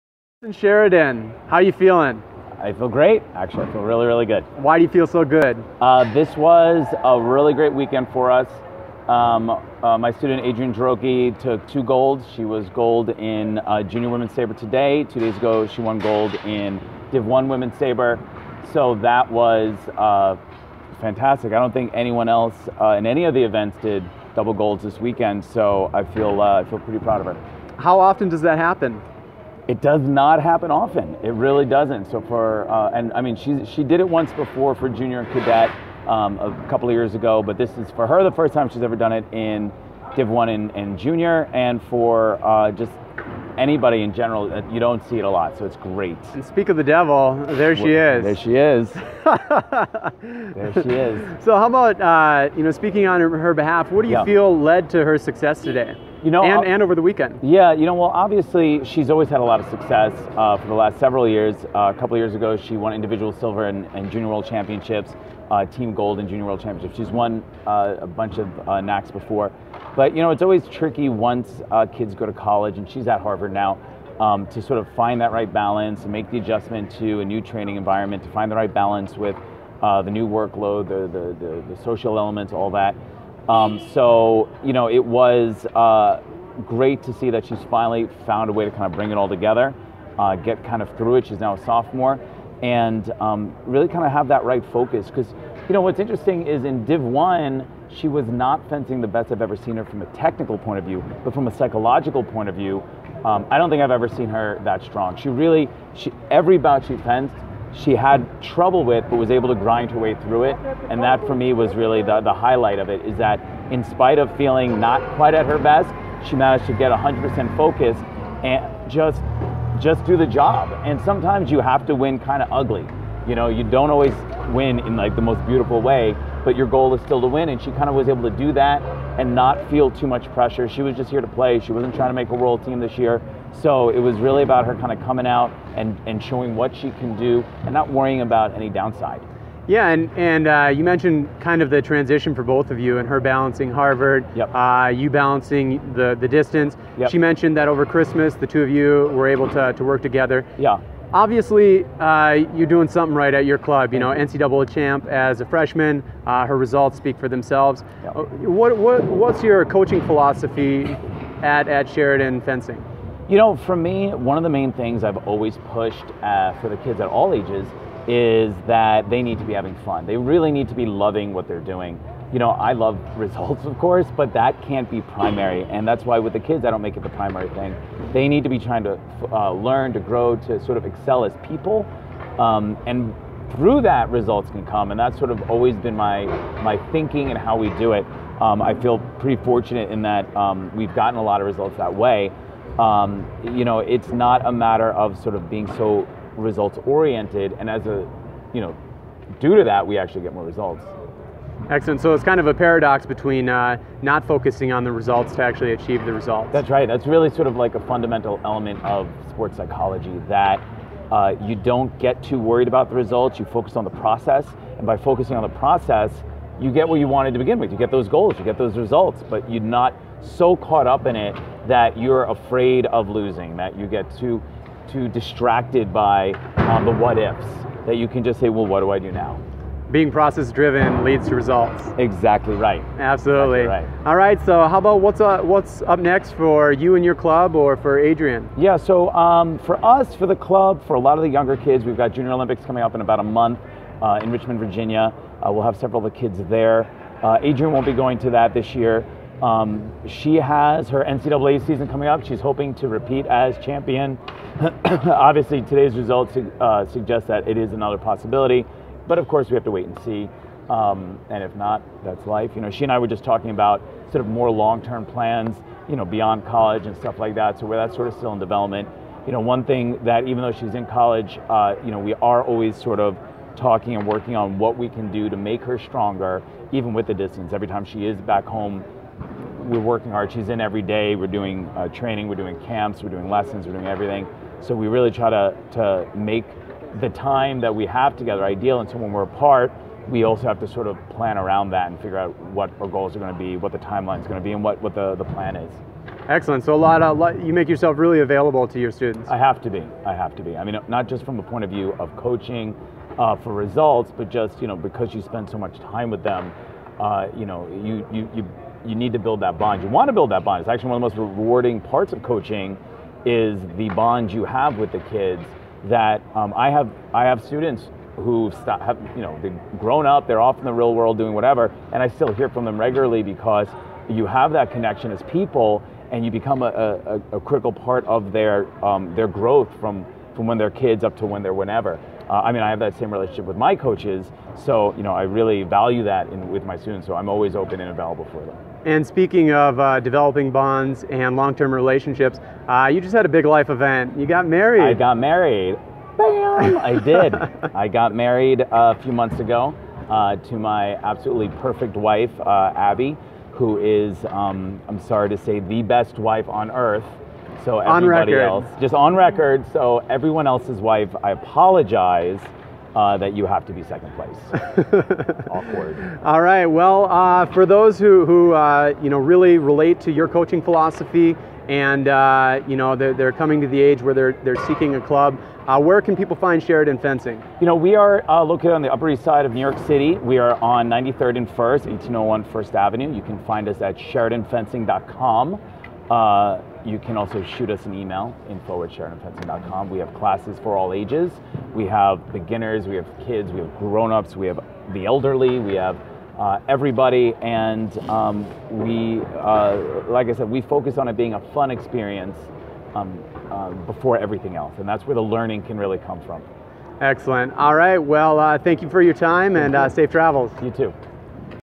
Sheridan Fencing: Process Driven to Get Results
Coach Jason Sheridan feeling "really really good" after his student, Adrienne Jarocki, takes home TWO golds from the January NAC in Salt Lake City.  Jarocki defeated Sarah Merza in the Senior Women's Sabre final Saturday before have winning again in their rematch with during the Junior Women's Sabre final Monday.